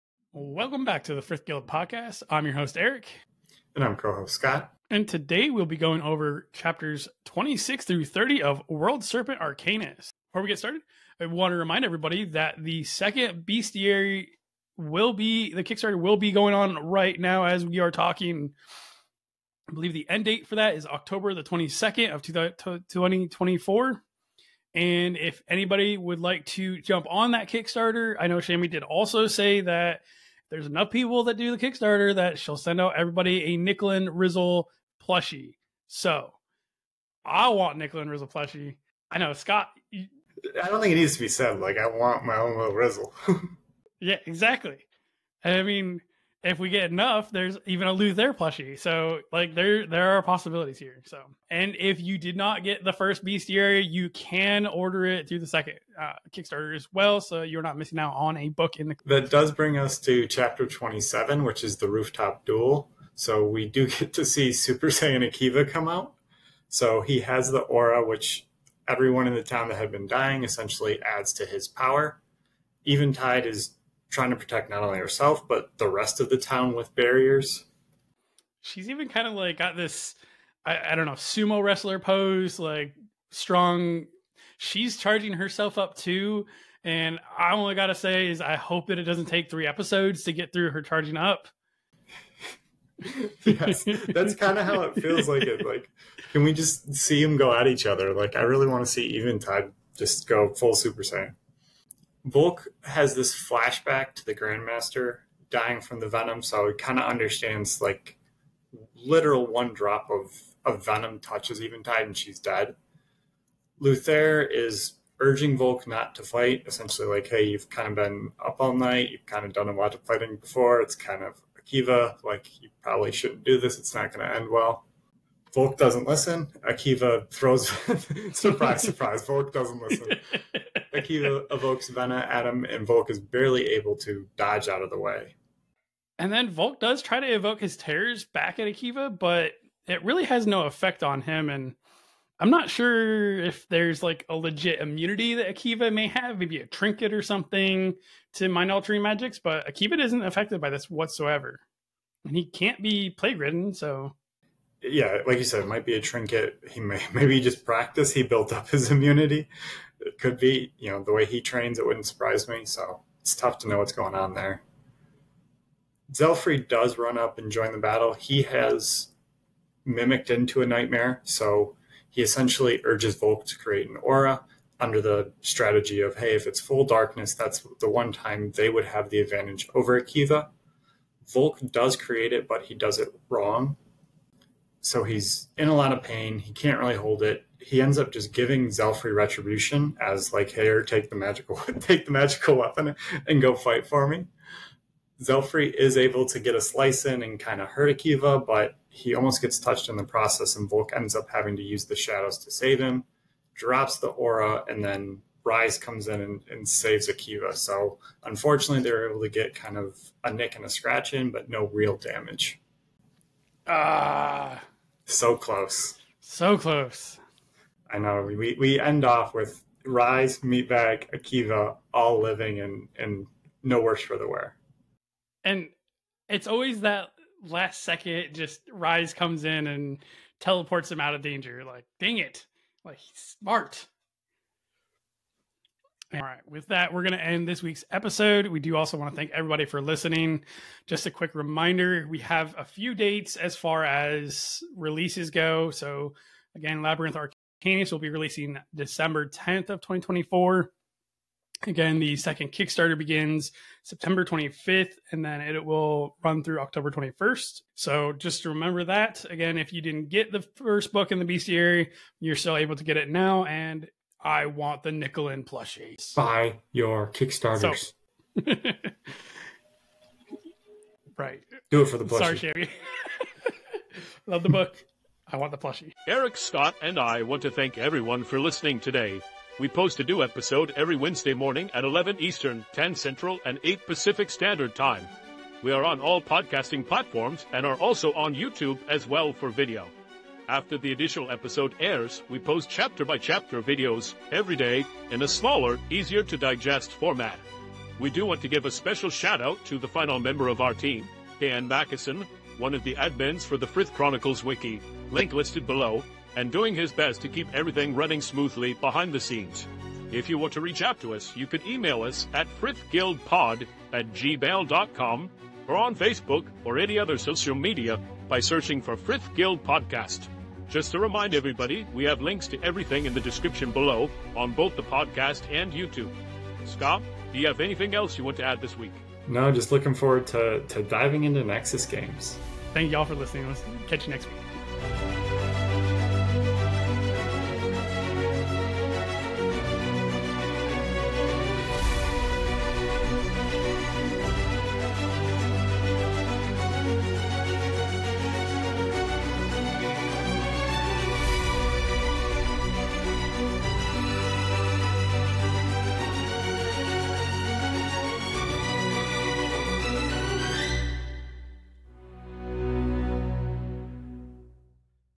High-Stakes Rooftop Duel: Akiva and Volke Face Off in 'World Serpent Arcanist' Chapter 27
Experience the electrifying 'Rooftop Duel' in Chapter 27 of 'World Serpent Arcanist.' Watch as Super Saiyan Akiva and Volke clash in a battle of venom, barriers, and mind-altering magic. Eventide's defenses, Zelfree's nightmare tactics, and Rhys' heroic intervention create an epic showdown. Don't miss this heart-pounding chapter that pushes our heroes to their limits.

I want to mention the Frith Chronicles Wiki as a great resource for fan art and information.
Thank you to Dan Mackison as the editor!

To Purchase Books on Amazon